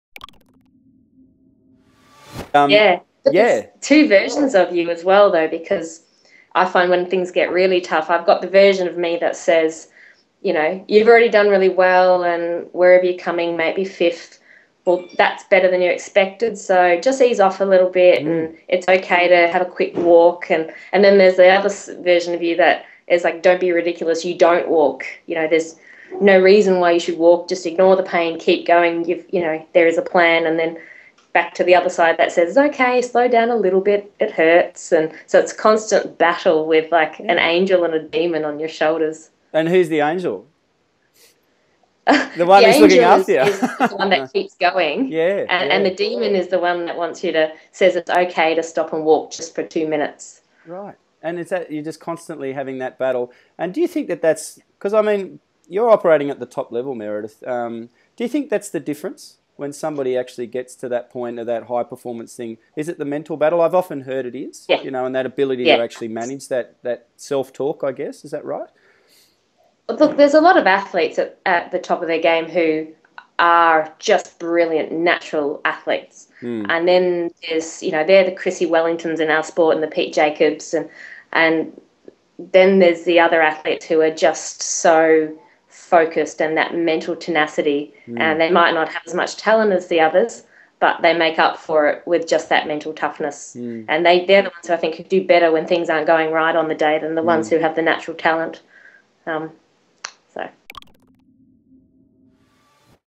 Meredith Hill Ironman Champion short
When it comes to endurance sport, it's all mental, and Change Chai Ambassador Meredith Hill knows what that means. Placing Top 10 in the the World Ironman Championships in Hawaii, Meredith spells out what it takes to mix it with best and be a "normal" Aussie as well.